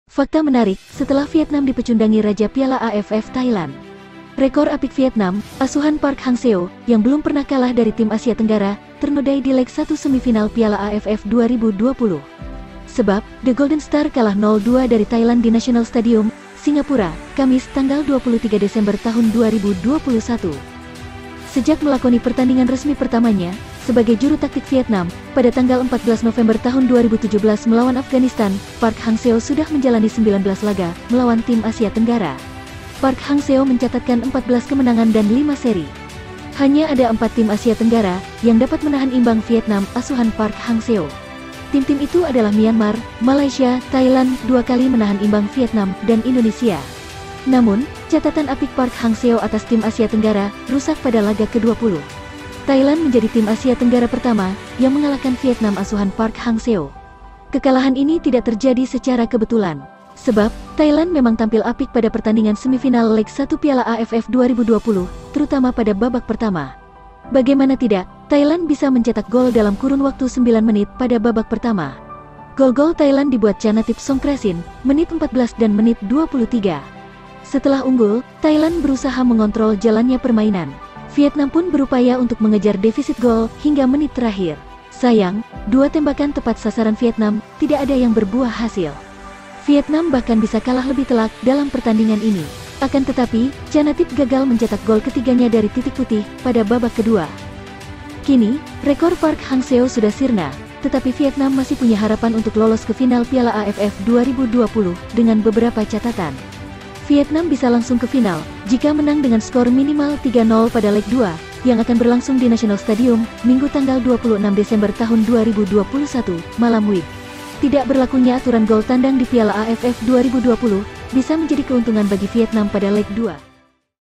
Fakta Menarik! Vietnam dipecundangi Raja Piala AFF Thailand | Semifinal Leg 1 | Piala AFF 2020
Fakta Menarik satelah Vietnam dipecundangi Raja Piala AFF Thailand | Semifinal Leg 1 | Piala AFF 2020
Sumber:

Rekor apik Vietnam,, asuhan Park Hang-seo, yang belum pernah kalah dari tim Asia Tenggara, ternodai di leg I semifinal Piala AFF 2020.
Sebab, The Golden Stars kalah 0 2 dari Thailand di National Stadium, Singapura, Kamis (23/12/2021).
Sejak melakoni pertandingan resmi pertamanya, sebagai juru taktik Vietnam, pada 14 November 2017 melawan Afgahanistan, Park Hang-Seo sudah menjalani 19 laga, melawan tim Asia Tenggara.
Park Hang-seo mencatatkan 14 kemenangan dan lima seri.
Hanya ada empat tim Asia Tenggara, yang dapat menahan imbang Vietnam, asuhan Park Hang-seo. Tim-tim itu adalah Myanmar, Malaysia, Thailand (dua kali menahan imbang Vietnam), dan Indonesia.
Namun, catatan apik Park Hang-seo atas tim Asia Tenggara, rusak pada laga ke-20. Thailand menjadi tim Asia Tenggara pertama, yang mengalahkan Vietnam asuhan Park Hang-seo.
Kekalahan ini tidak terjadi secara kebetulan. Sebab, Thailand memang tampil apik pada pertandingan Semifinal Leg 1 Piala AFF 2020, terutama pada babak pertama.
Bagaimana tidak, Thailand bisa mencetak gol dalam kurun waktu sembilan menit, pada babak pertama. Gol-gol Thailand dibuat Chanathip Songkrasin (menit 14’dan menit 23’).
Setelah unggul, Thailand berusaha mengontrol jalannya permainan. Vietnam pun berupaya untuk mengejar defisit gol, hingga menit terakhir.
Sayang, dua tembakan tepat sasaran Vietnam, tidak ada yang berbuah hasil. Vietnam bahkan bisa kalah lebih telak, dalam pertandingan ini.
Akan tetapi, Chanathip gagal mencetak gol ketiganya dari titik putih, pada babak kedua.
Kini, rekor Park Hang-seo sudah sirna, tetapi Vietnam masih punya harapan untuk lolos ke final Piala AFF 2020, dengan beberapa catatan.
Vietnam bisa langsung ke final, jika menang dengan skor minimal 3 0 pada leg II, yang akan berlangsung di National Stadium, Minggu 26 Desember 2021, malam WIB.
Tidak berlakunya aturan gol tandang di Piala AFF 2020, bisa menjadi keuntungan bagi Vietnam pada leg II.